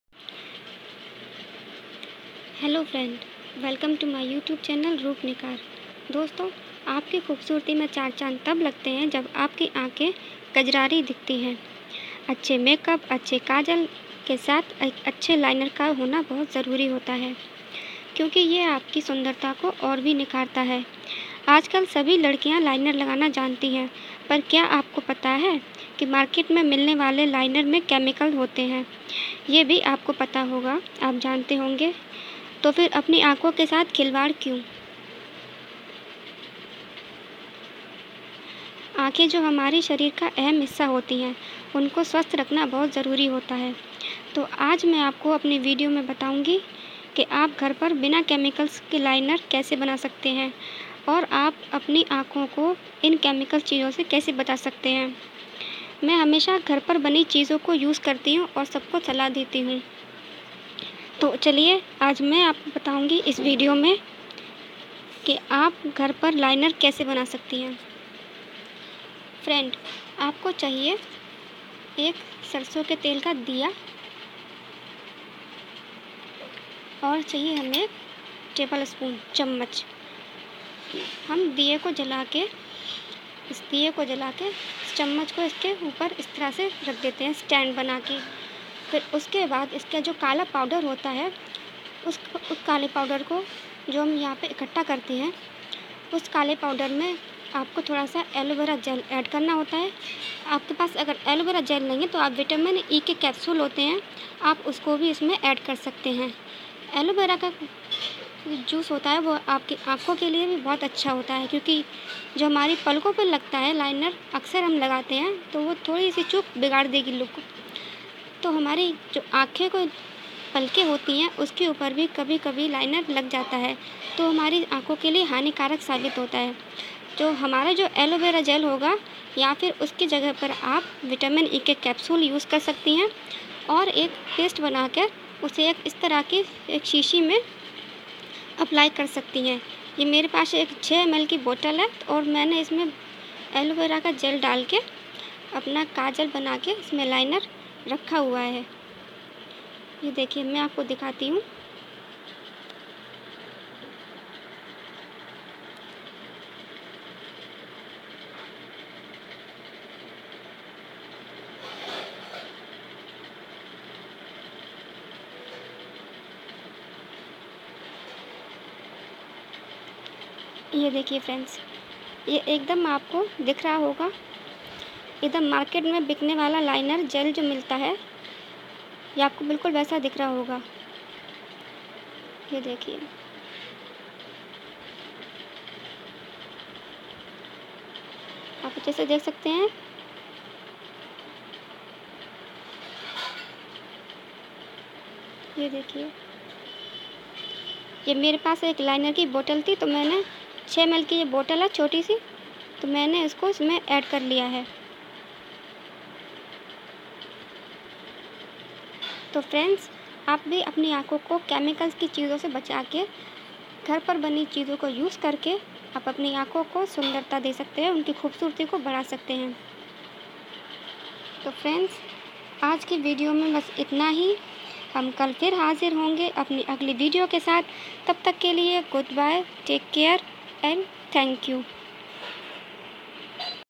घर पर काजल केसे बनिये How to make mascara at home! !
hi guys my name is Jyoti.                                    एलो वेरा जेल से बाना kajal  इलाइनर जिस्को| घर पर एकदूम ताजा बीना केमिकल बीना आंखें को नुक्सान| पाहुंचता  मेरा वीडियो में आपको मिलेगा और भी जानकारी  शुक्रिया